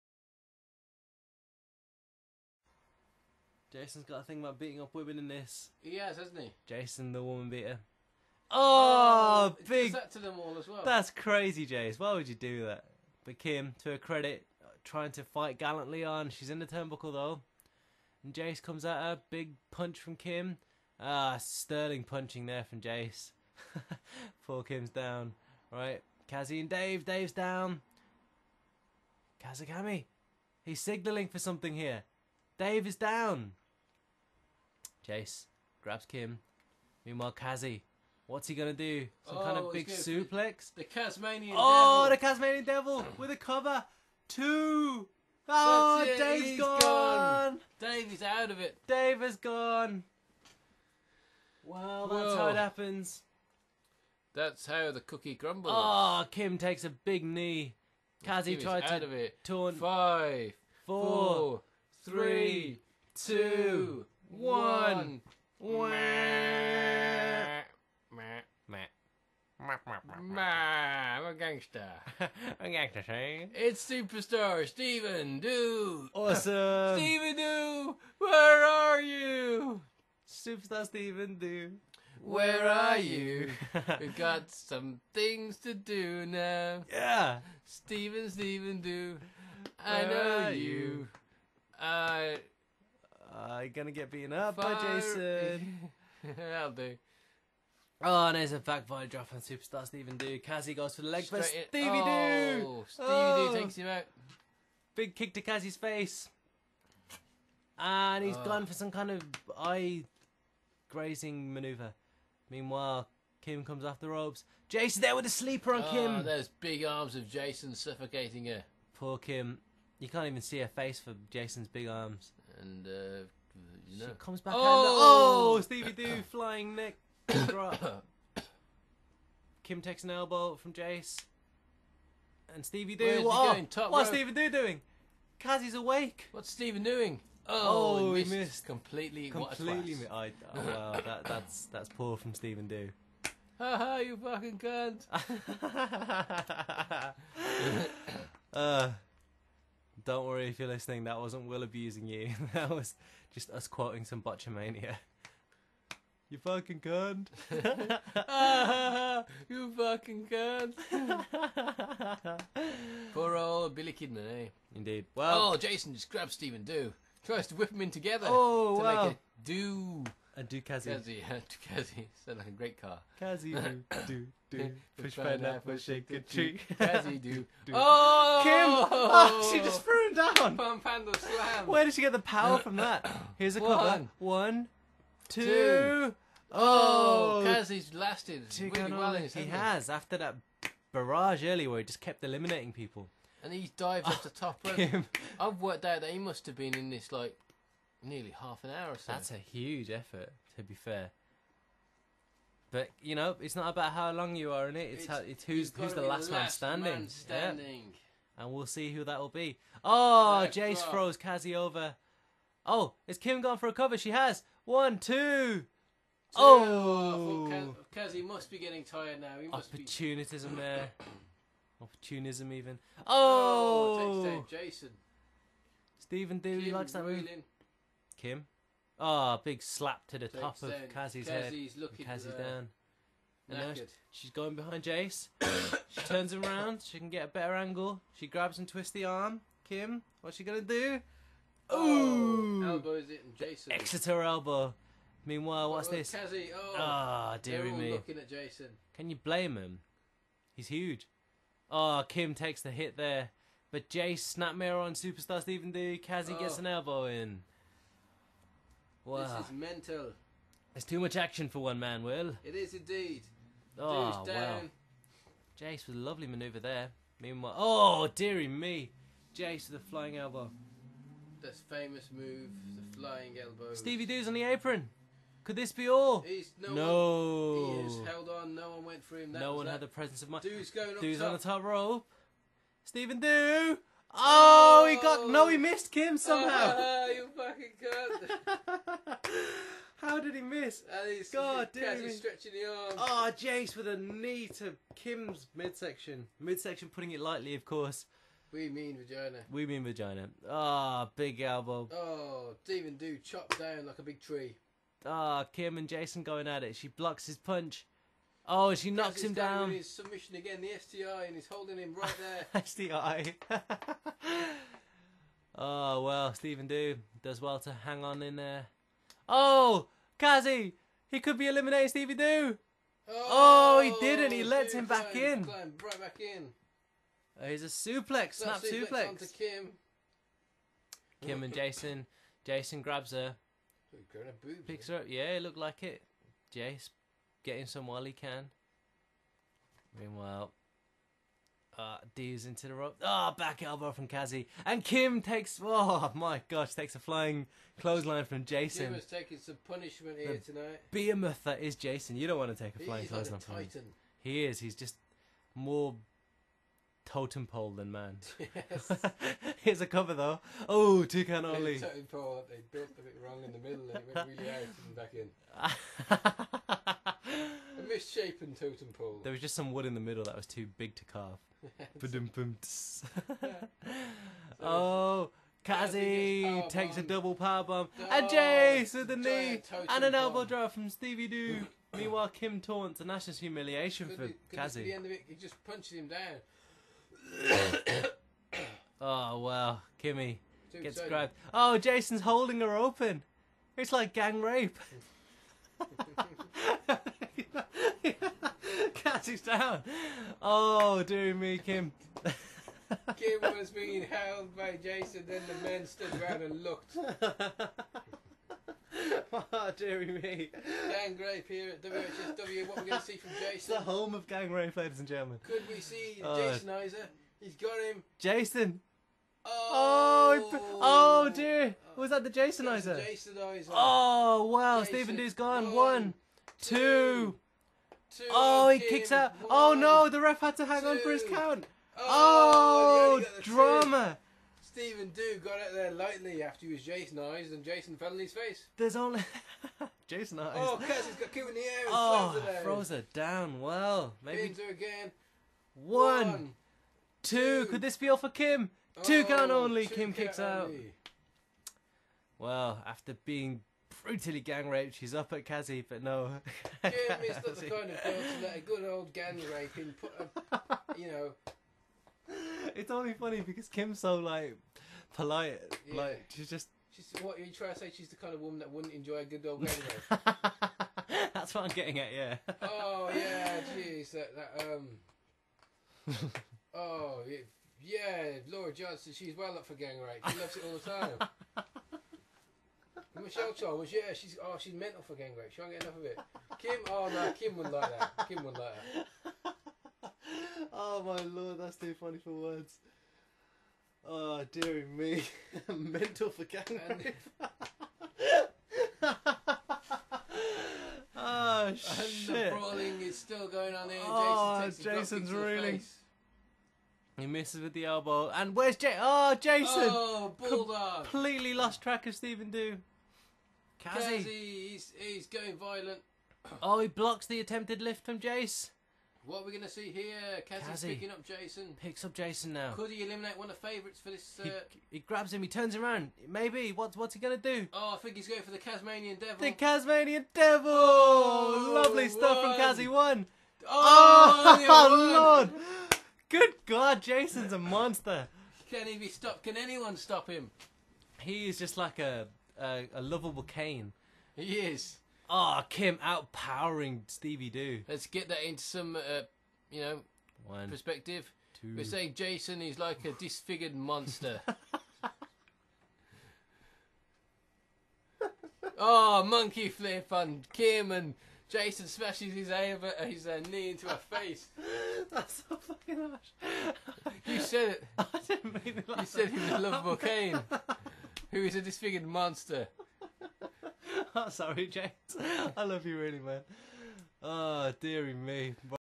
WHSW Royal Rumble prt8
40 Participants will enter! Only one will remain standing at the end!

The Winner of our Royal Rumble will earn themselves a title shot against our reigning  WHSW Heavyweight Champion - Iron Ian Shingler!